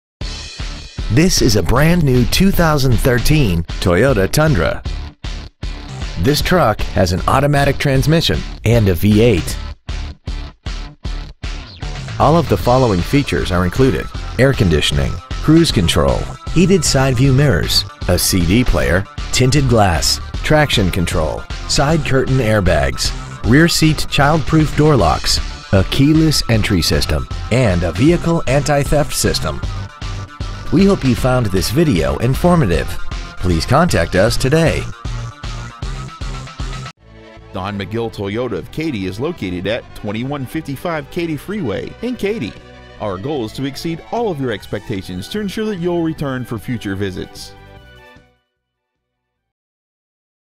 2013 Toyota Tundra Katy TX
Year : 2013
 Make : Toyota
 Model : Tundra
 Engine : 8 Cyl.
 Trans . : Automatic
 Exterior : Magnetic Gray Metallic

 Interior : Graphite
 Stock : K34612

 2013 Katy TX